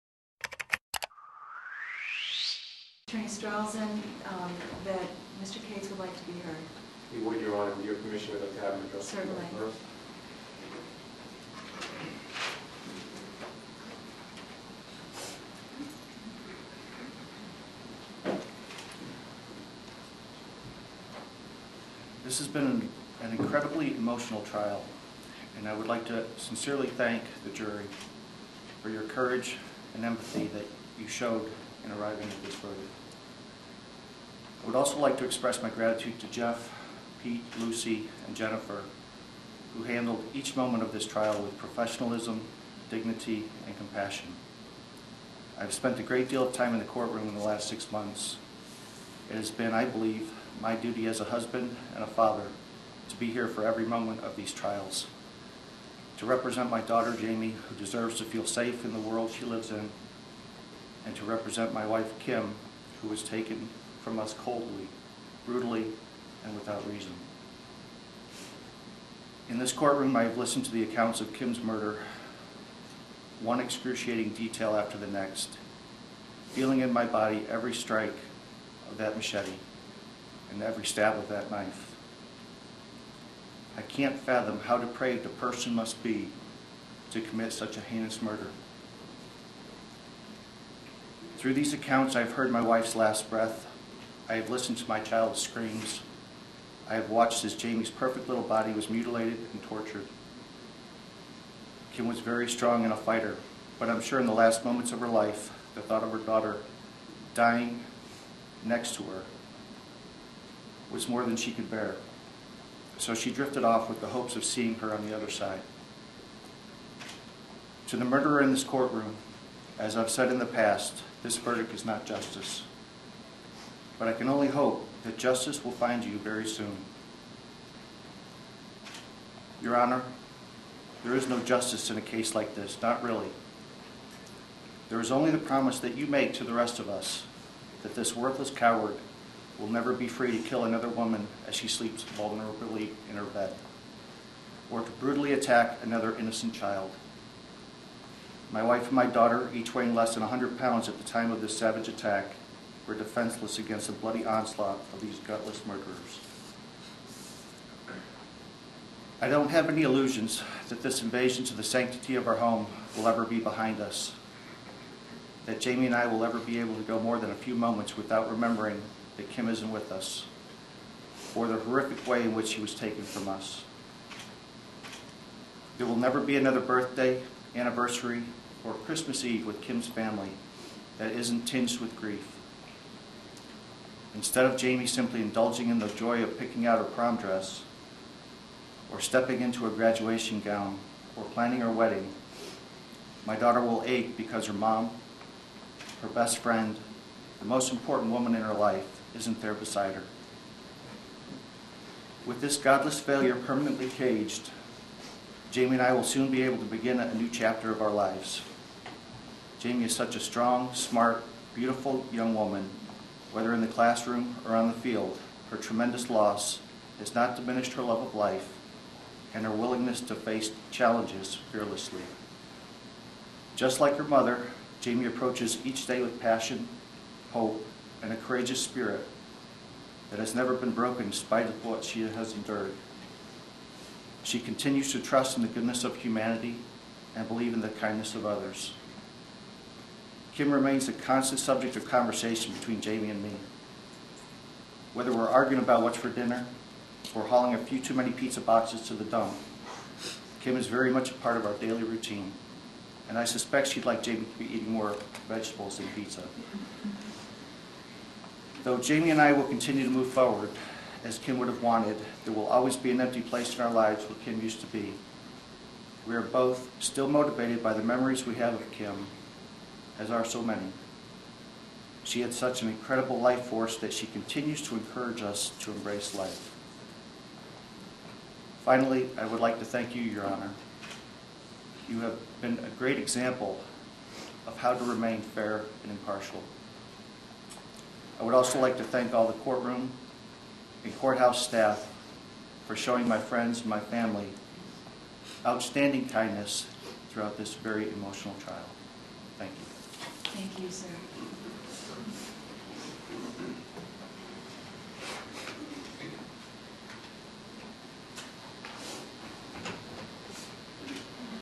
Uncut Video: David Cates Reads Victim Impact Statement
David Cates read a victim impact statement after Christopher Gribble was found guilty in the murder of his wife.